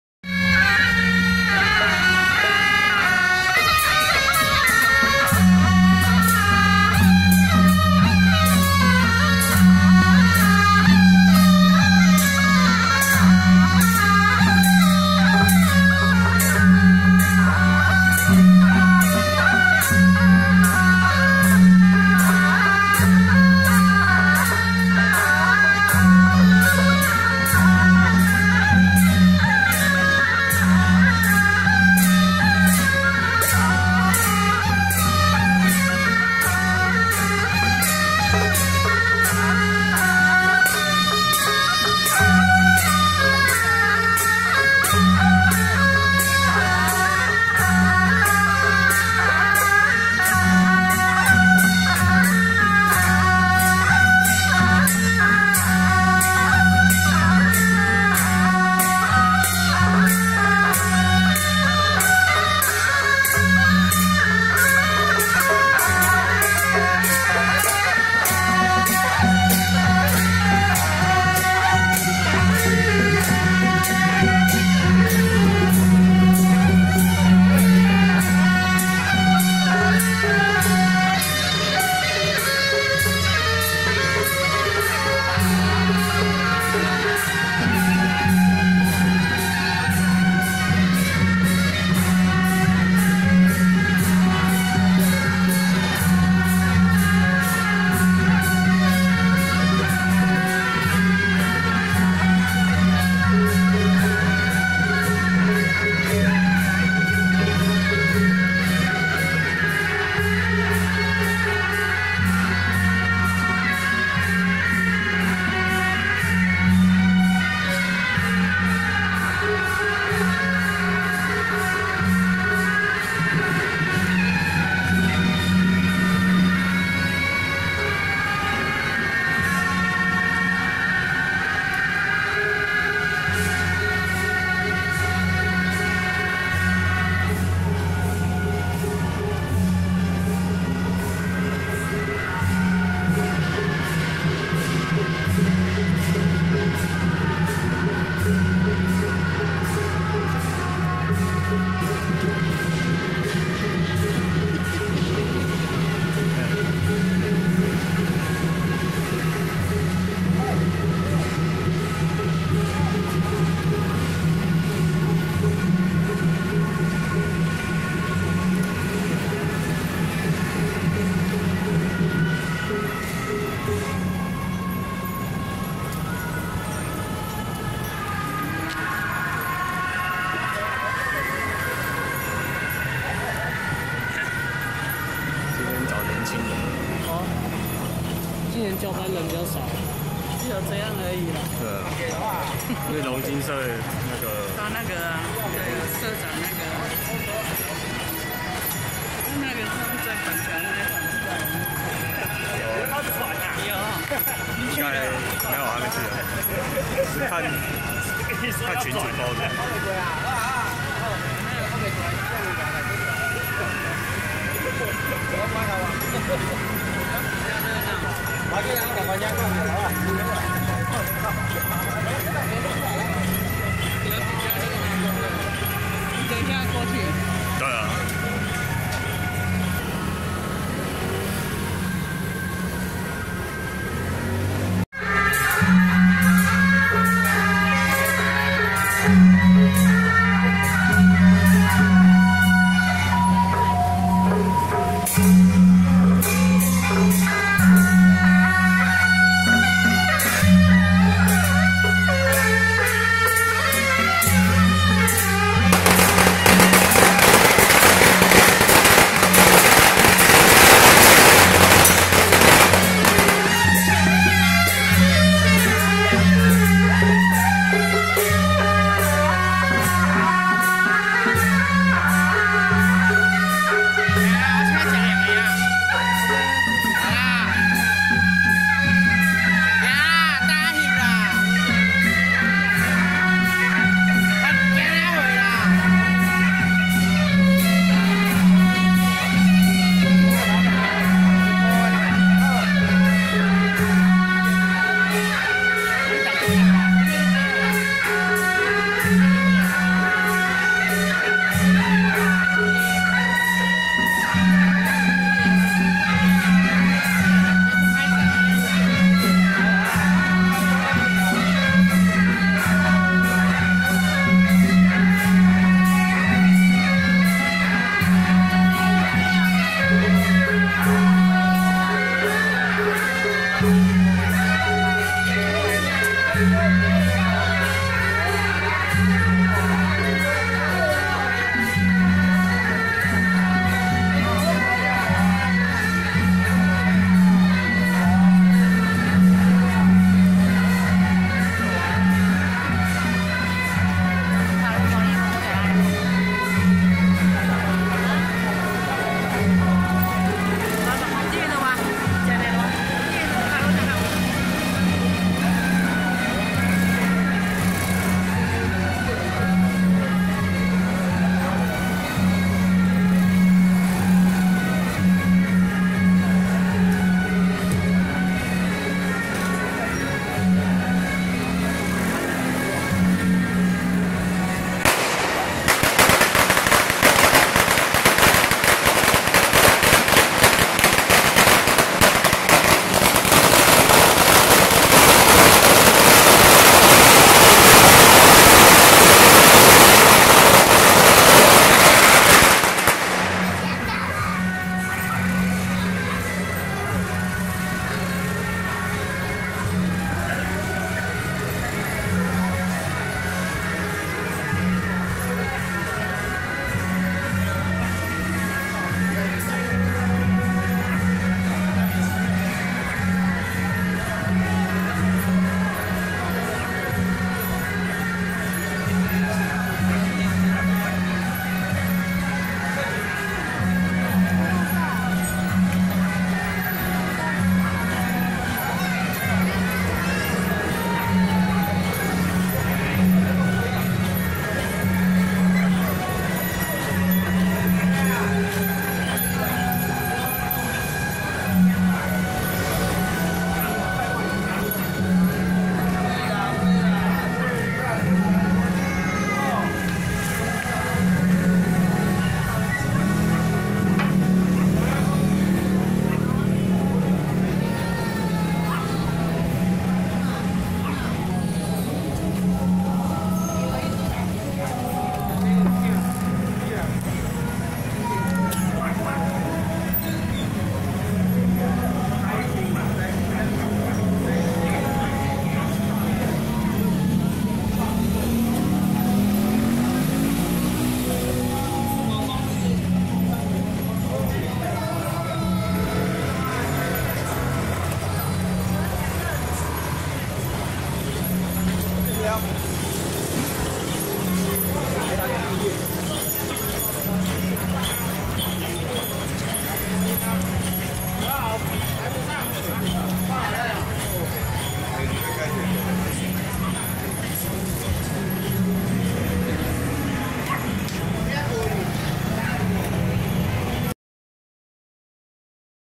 107艋舺龍津宮恭祝順正府大王公聖誕遶境【艋舺龍津宮-神將團＆順正府大王公 聖駕(台北穎華軒轎班團)】
艋舺龍津宮-神將團＆順正府大王公 聖駕(台北穎華軒轎班團)